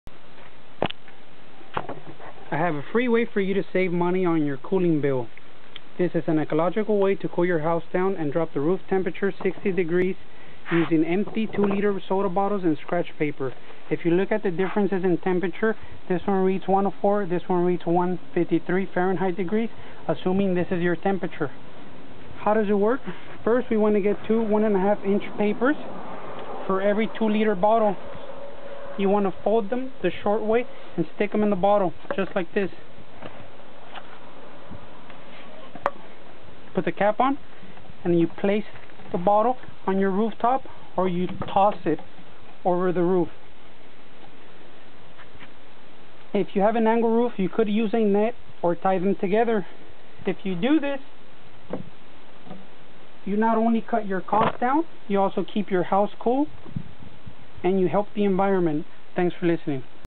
cool roof.MOV
a green way to cool off your roof and cut your cooling costs.  This really works and it is free.    In the summer put two liter soda bottles on your roof with two pieces of paper in them.    this can cut the temperature on your roof by over 60 degrees that we simulate here.    we put a temperature probe under a dark roof in two places.   You can see the difference that using free two liter bottles with recycled paper can do.     Ideally his needs to be two layers to beat having a white roof.     this saves space in the landfills as well as cuts your use of electricity.     In the winter just stand the bottles up and the sun can then get through and warm the roof.    You can use a net or just tie the bottles together to keep them in place.